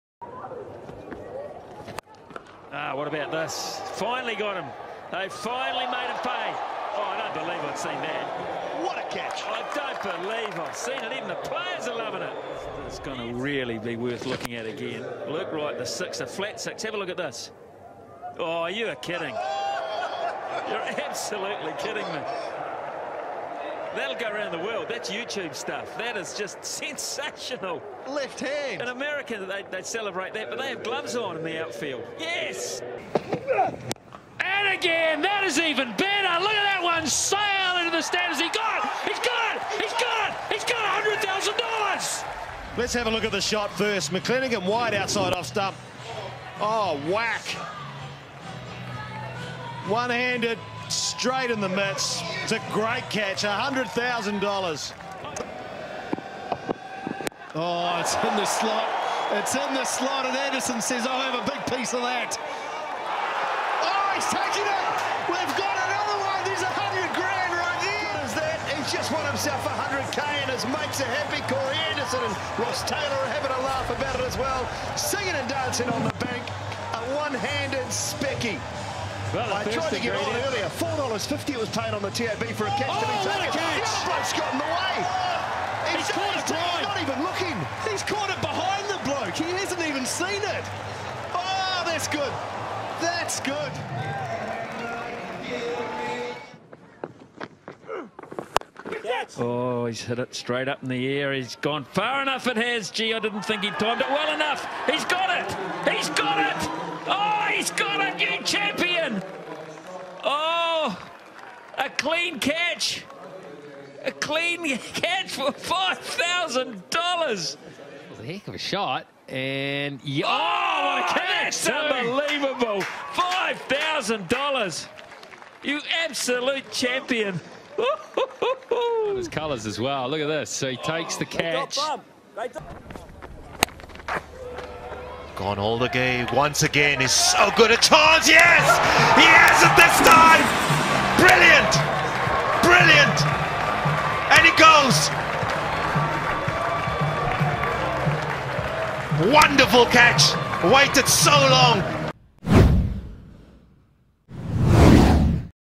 Amazing $5000 One Handed Catch by a Cricket Fan (Unbelievable)
Fan got $5000 for an amazing catch from the gallery. Along with that, we have compiled some more wonderful catch by super cricket fan from around the world. Enjoy!